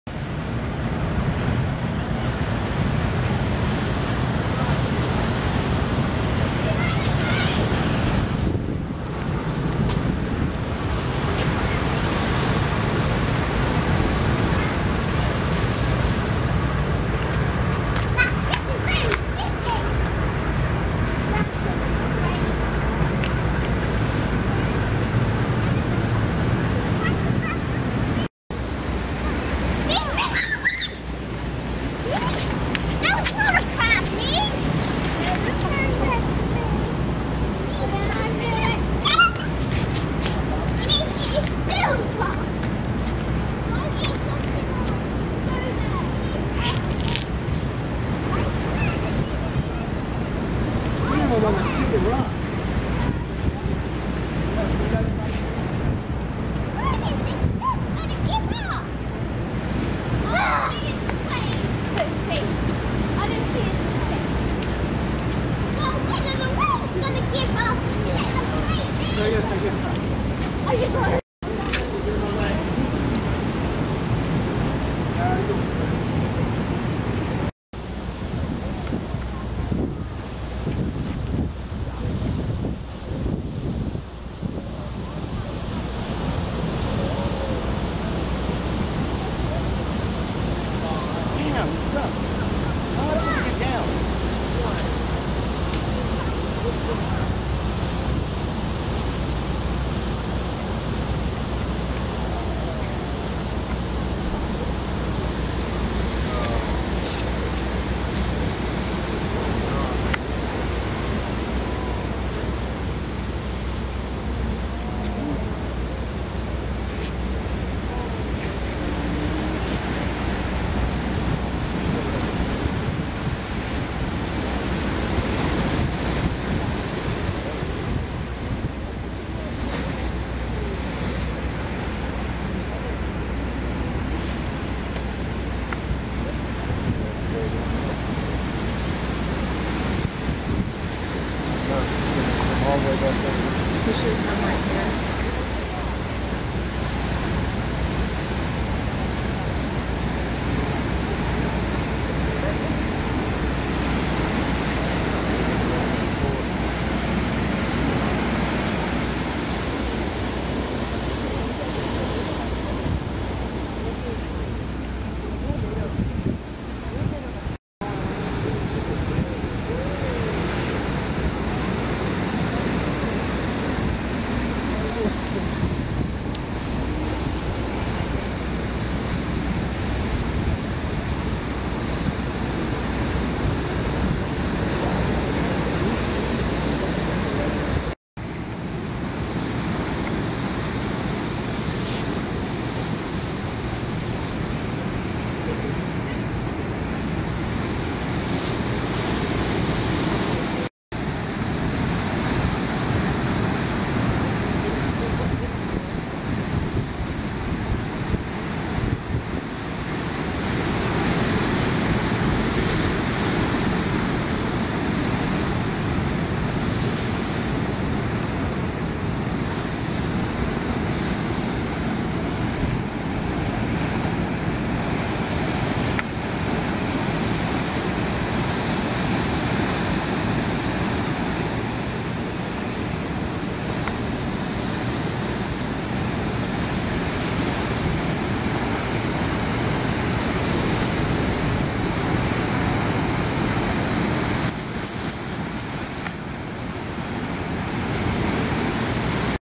Fishing from Cliff House or catching fish San Francisco style
Fishermen explained to me that they cannot bring the fish up because of all the rocks the line gets tore, so instead they are going down to get their prize. As you see it is not an easy task.